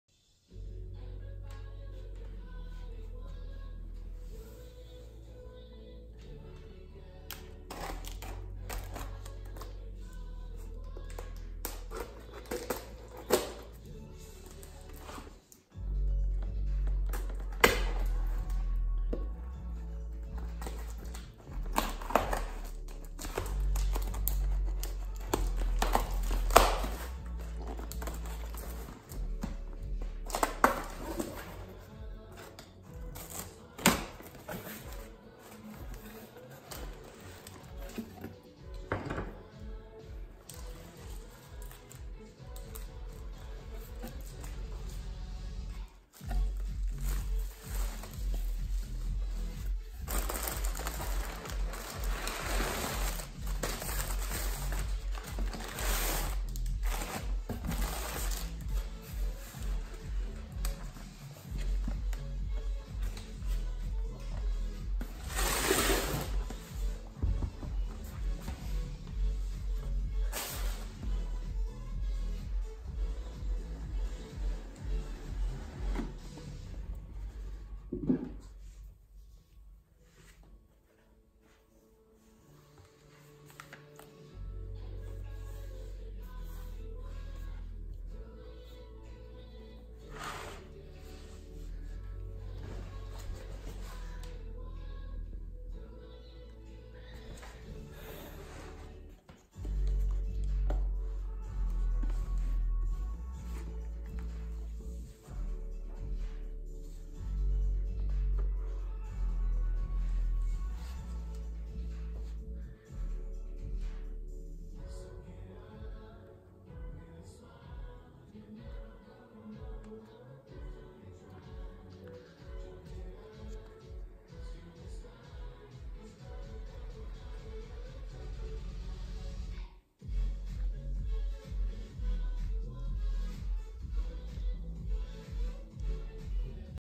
L3.774.4.50.0 Unbox
Longines Legend Diver L.888.2 movement based on ETA 2892-A2. 25200 bph and 64 hours power-reserve.